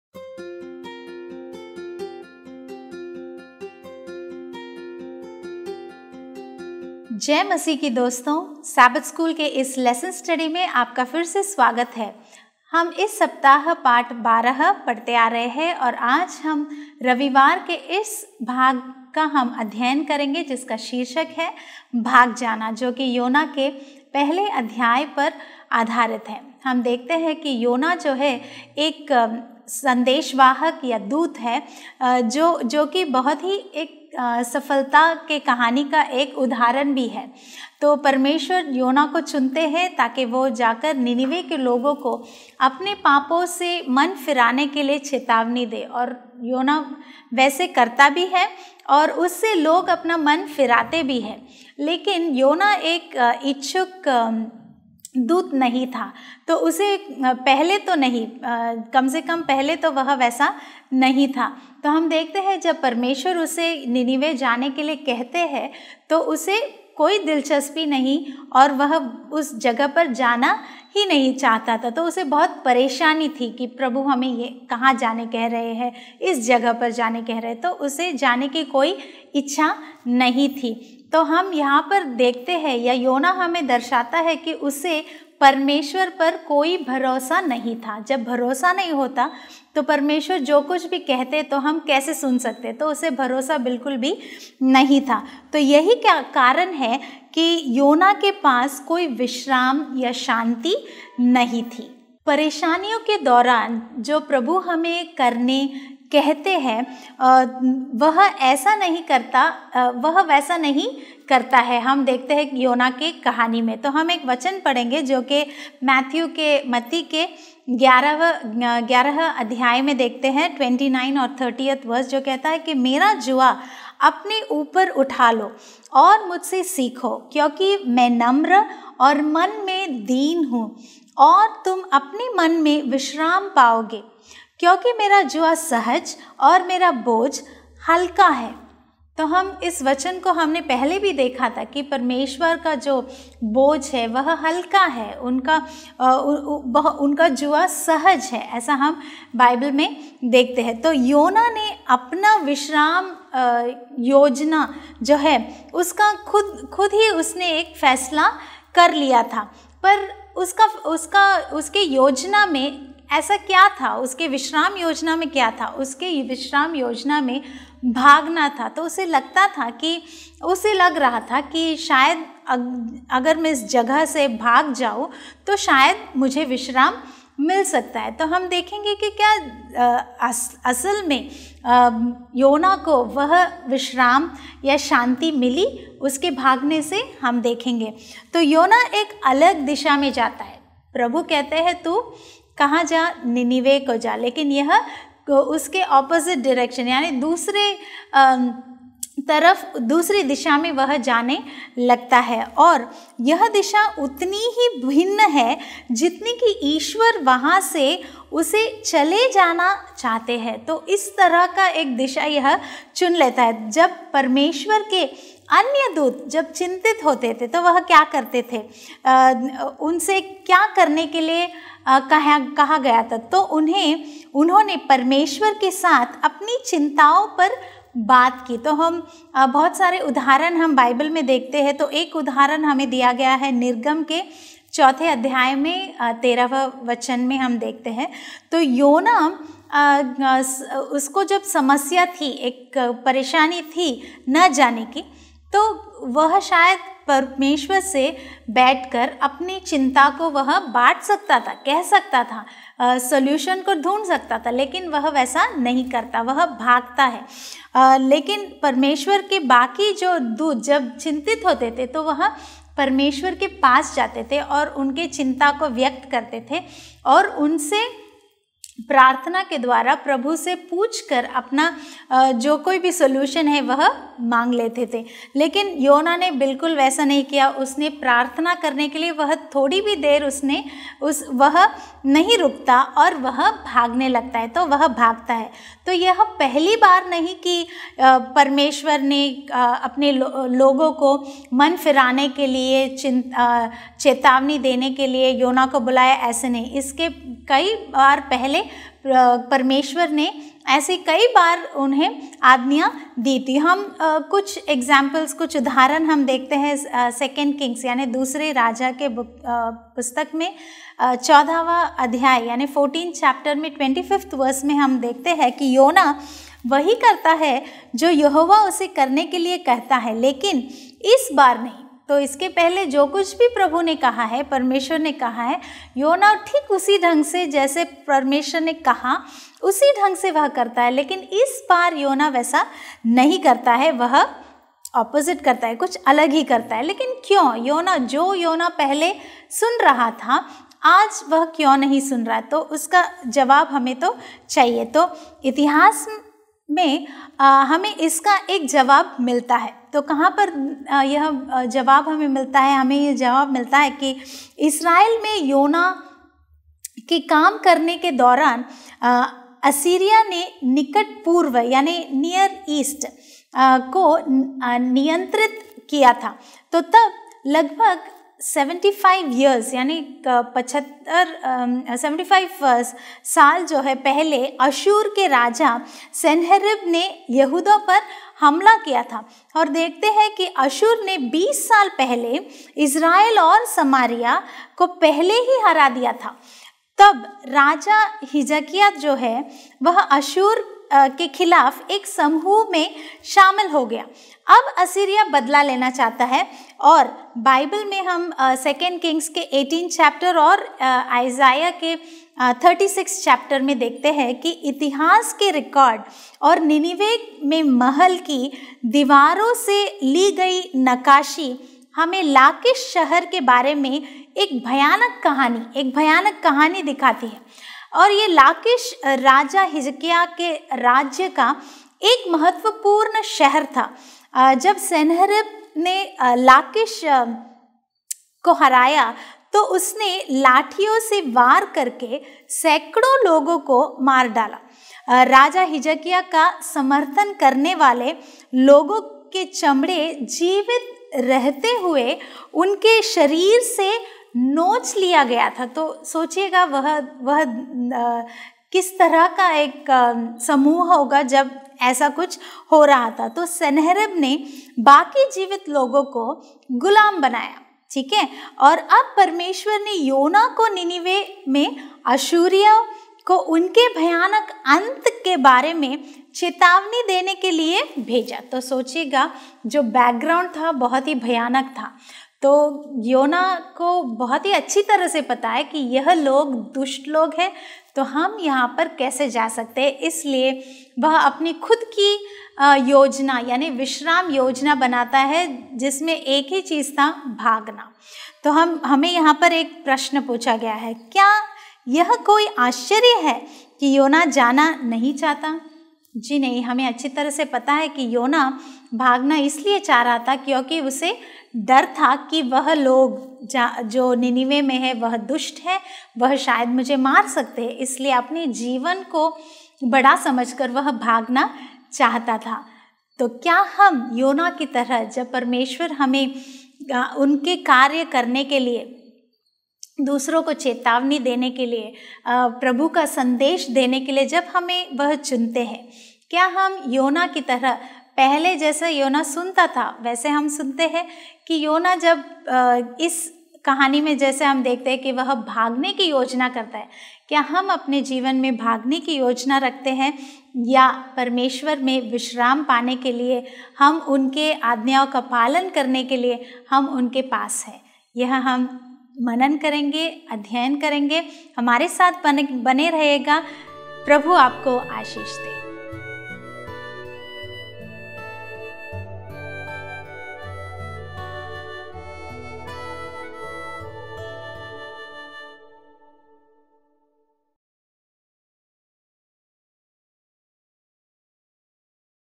LLBN SA | Sabbath School Lesson Hindi | विश्राम और परिवार | Q3 पाठ 12 | रविवार | Priya Charles
Adult Sabbath School, Quarter 3, 2021 (Hindi)
Lesson 12 | September 12 (Sunday)
Speaker: Priya Charles